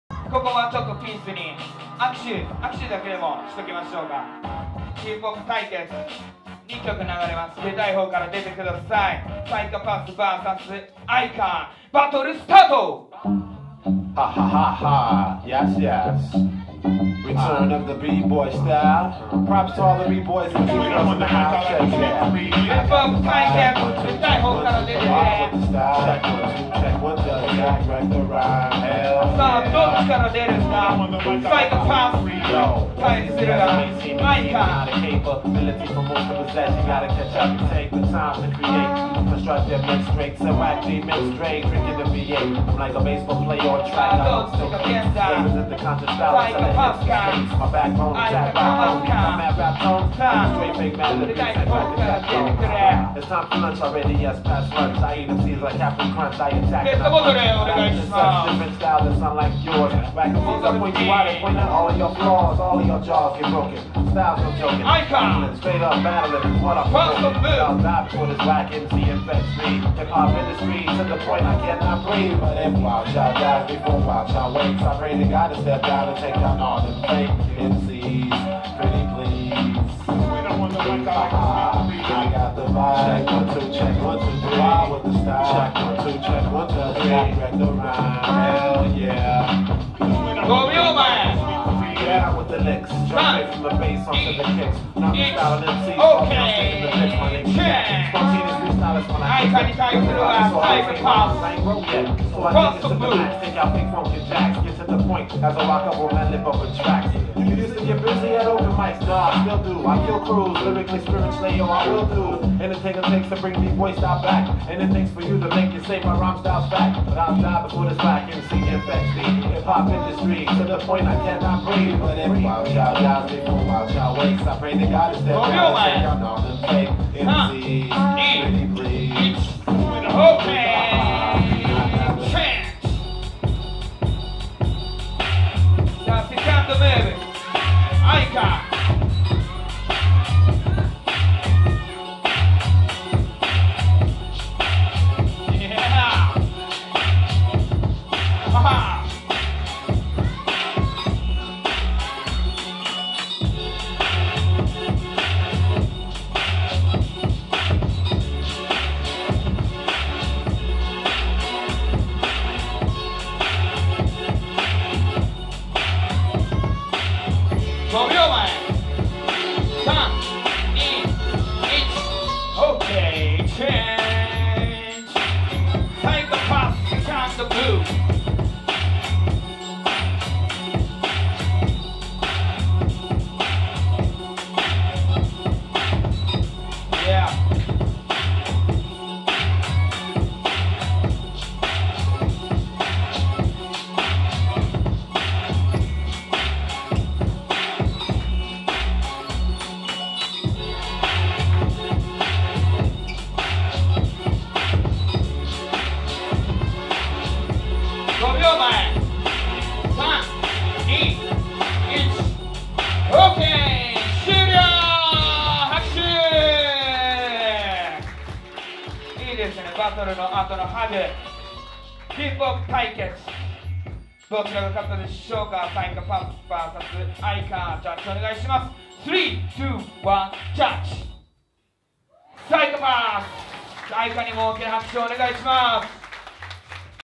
AIKA vs Sayakapus 一回戦_02(Kids) LOOP DE DANCE 2013.09.08 | UGcrapht×Beat Connection
LOOP DE DANCE - 5th Season - Vol.5

JUDGE
・N@O (Ziel) from 仙台
・だいすけ (The Uptown Clan / Flying Beat BBoyz / Ekusaid) from 山形
・ちゃんなつ (お～でぃんず) from 山形

DJ
・DJ 辰 (UGcrapht)
・DJ DAISUKE

MC
・DELOREAN M.I.C (C.O.X. & President K)